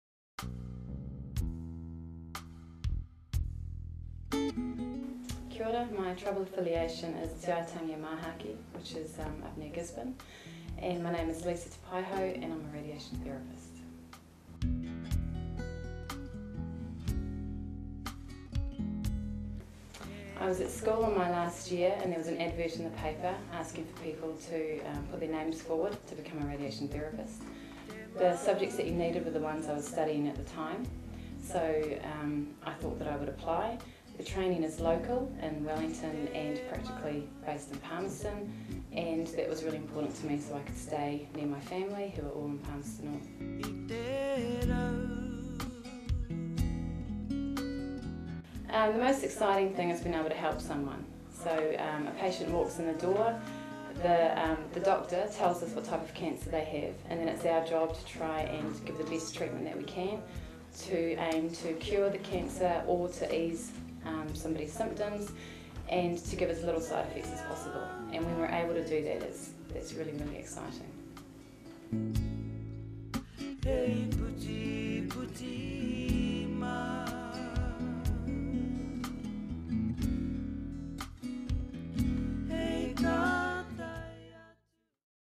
MDHB Maori Workforce Development - radiation therapist
Experience of a Maori radiation therapist working at MidCentral District Health Board